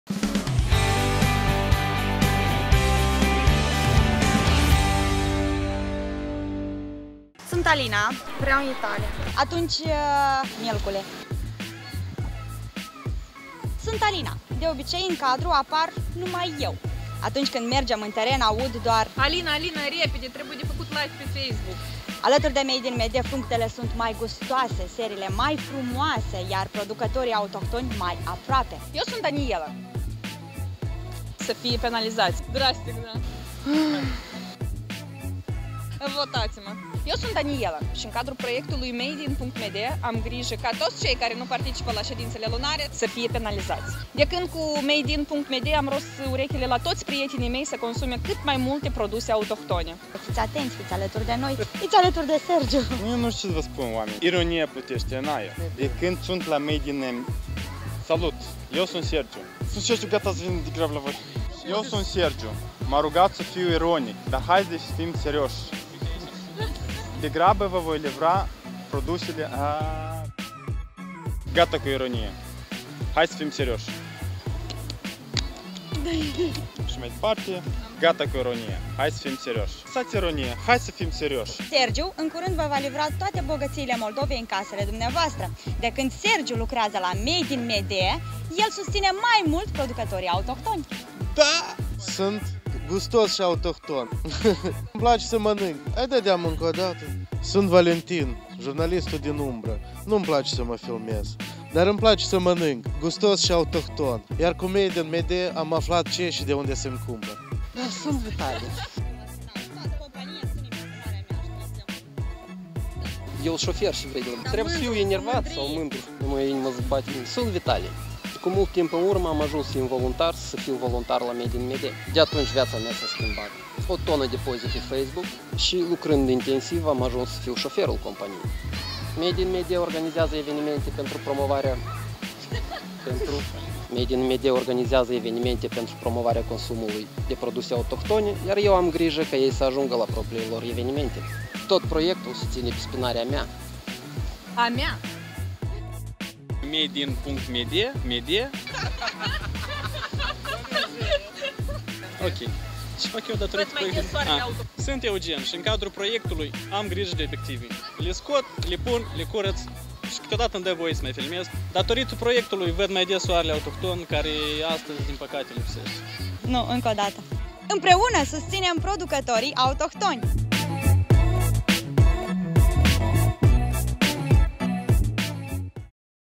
Echipa Madein.md
Madein.md reprezintă un proiect de promovare a produselor fabricate în Moldova și de susținere a producătorilor autohtoni.